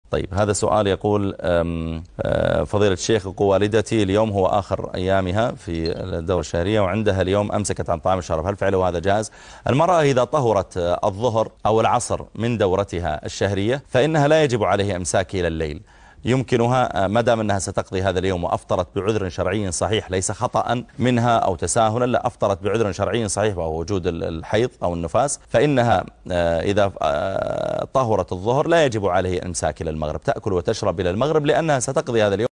حكم إفطار المرأة إذا انتهت الدورة الشهرية نهاراً
الفتاوى لفضيلة الشيخ : محمد العريفي